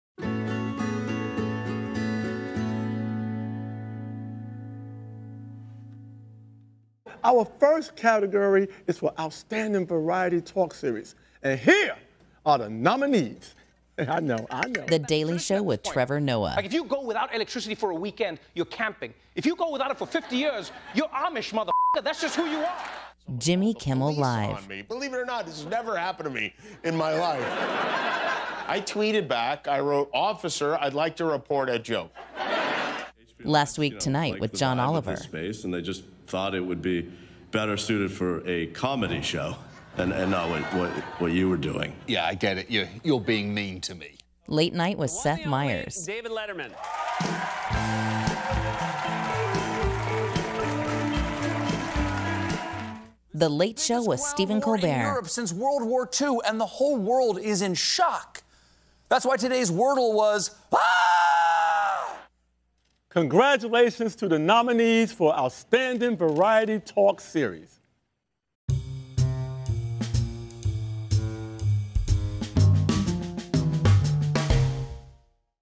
74th Emmy Nominations: Variety Talk Series
The nominees for Outstanding Variety Talk Series are The Daily Show with Trevor Noah, Jimmy Kimmel Live!, Last Week Tonight with John Oliver, Late Night with Seth Meyers, and The Late Show with Stephen Colbert.